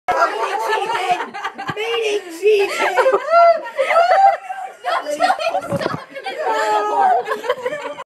It's mating season
So this was taken back in October 2012 with Jillian's bunny puddles and mine and Winston's bunny charlet